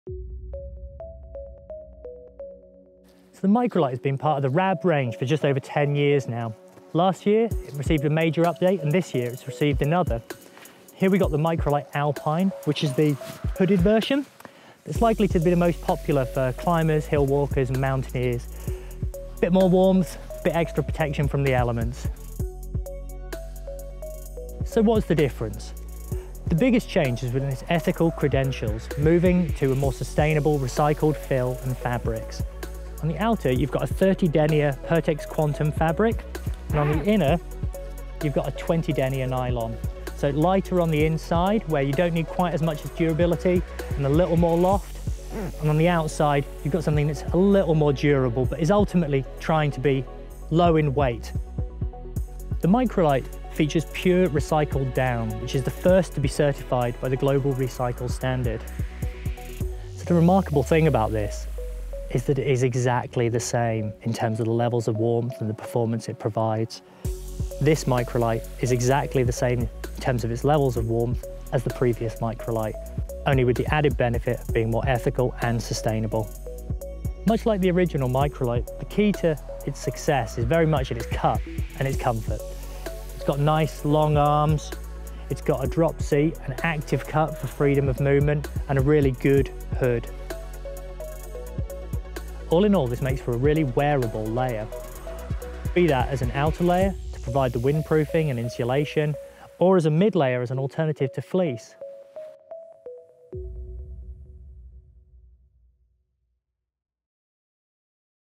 The Rab Microlight Alpine - Made for Climbers, Hillwalkers and Mountaineers
Rab's classic lightweight down jacket has been updated. With a fresh design aesthetic and recycled construction, the highly packable Microlight Alpine Jacket is the go-to system for rapid warmth when the temperature drops.